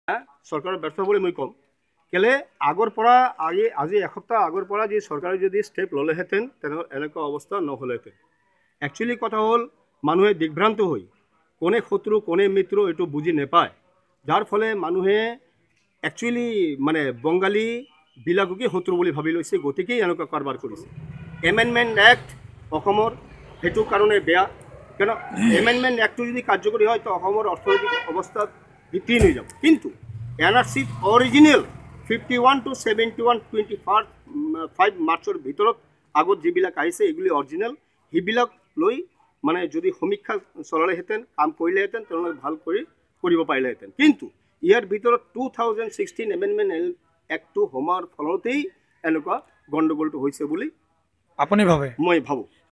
নাগৰিকত্ব আইন অসম চৰকাৰৰ ভুল সিদ্ধান্ত : জনসাধাৰণ
নাগৰিকত্ব সংশোধনী বিধেয়কক চৰকাৰৰ ভ্ৰান্তিমূলক কাৰ্য আখ্যা সাধাৰণ জনতাৰ। ইটিভি ভাৰতৰ সাংবাদিকৰ ওচৰত এন আৰ চি সন্দৰ্ভত মুখ খুলিছে লামডিঙৰ সুজিত কুমাৰ ঘোষ নামৰ ব্যক্তিএগৰাকীয়ে।